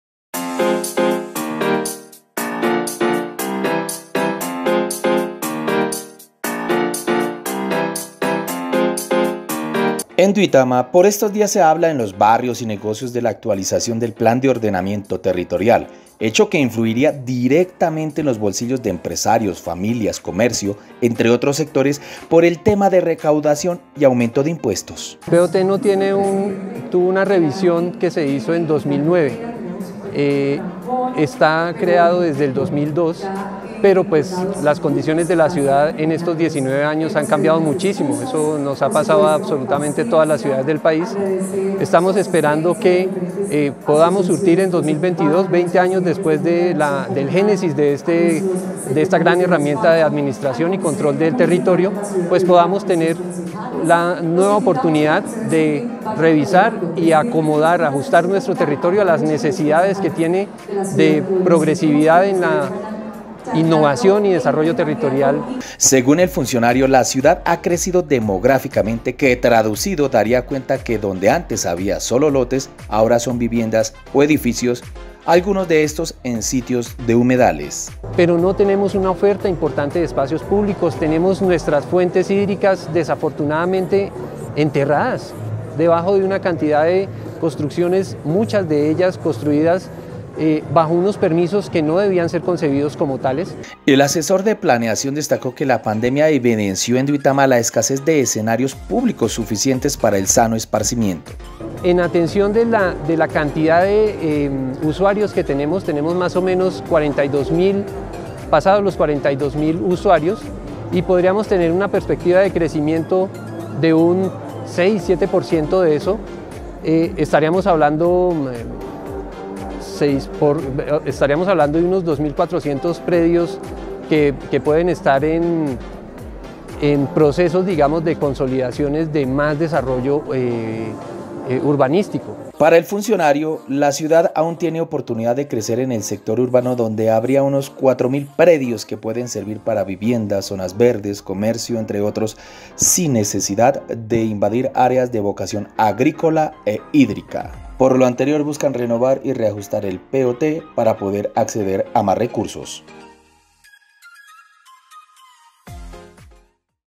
TopNoticioso1 A LA ‘CAZA’ DE IMPUESTOS CON EL POT
Cuando el comercio, familia y empresarios oyen hablar de la renovación y reajuste del POT de su ciudad, lo primero que piensan es en que deben pagar más impuestos. Duitama quiere renovar este documento.